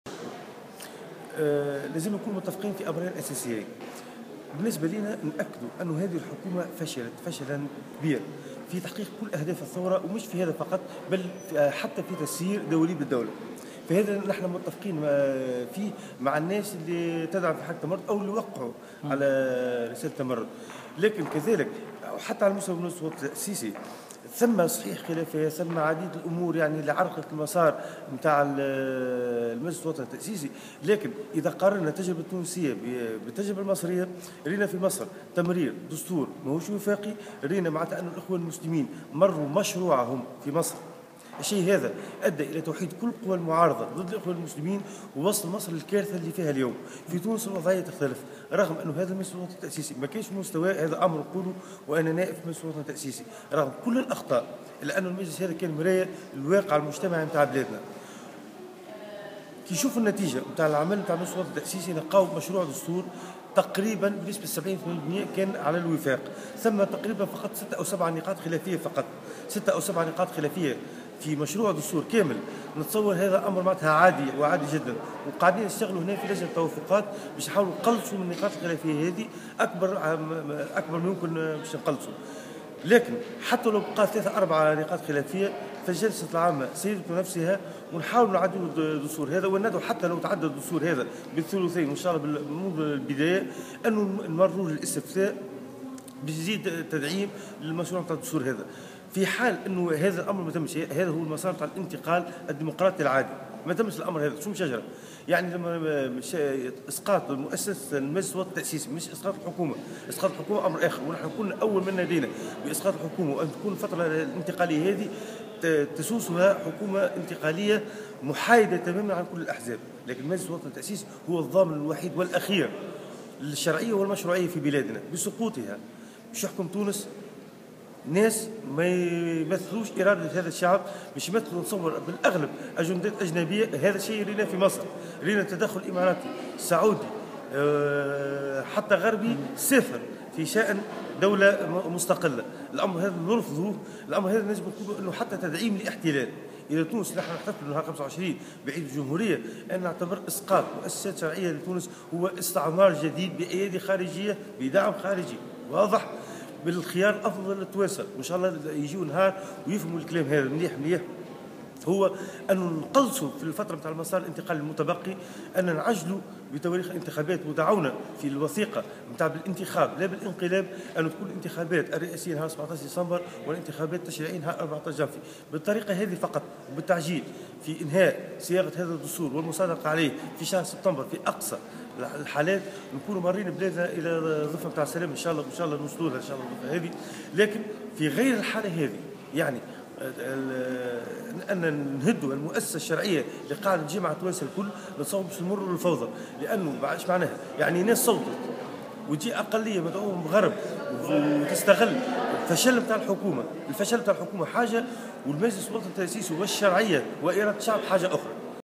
اسكندر بو علاق مستقبل تونس بعد الثورة
اسكندر بو علاق من حزب العريضة الشعبية و عضو المجلس التأسيسي يتحدث عن مستقبل تونس السياسي
Skander Bouallègue membre de l'Assemblée constitutanete et membre du parti Al Aridha nous parle de l'avenir politique de la Tunisie
Vister pour laisser un commentaire